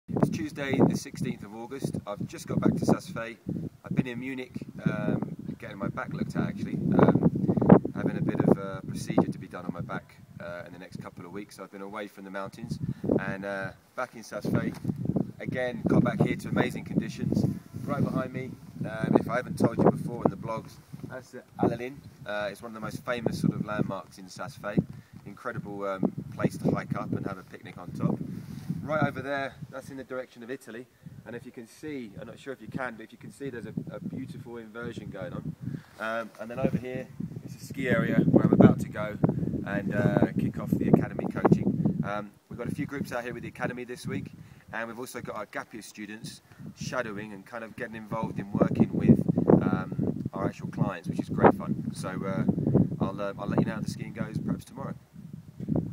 Warren Smith - 16th August 2011 Blog - Saas-Fee.MOV
Warren has just got back from Saas-Fee after getting some treatment on his back. Not to his surprise the glacier is in amazing condition.  This week the GAP year students are shadowing and teaching with some of the groups on the Academy weeks.